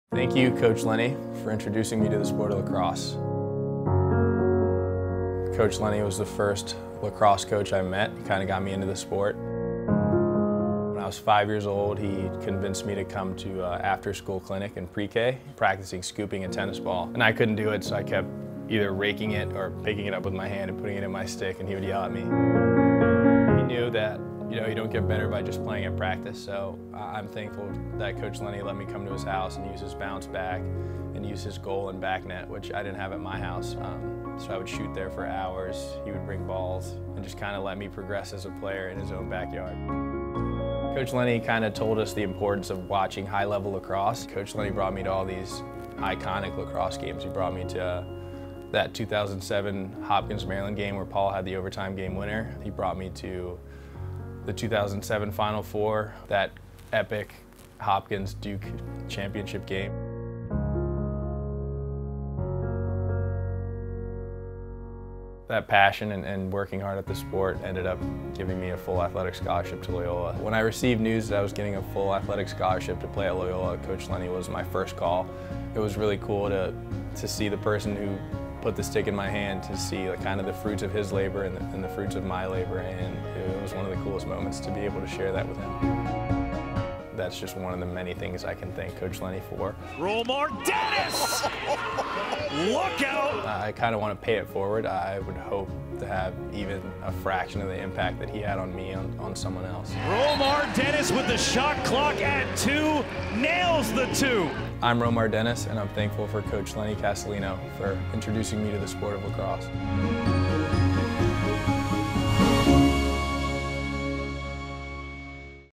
Romar Dennis
Who are you thankful for in lacrosse?

For Romar Dennis, it’s Coach Lenny, who first put a stick in his hands when he was 5 & inspired his love for lacrosse by taking him to Hopkins games.

As we celebrate Thanksgiving, share your story & use ThankYouLacrosse to be featured 🙏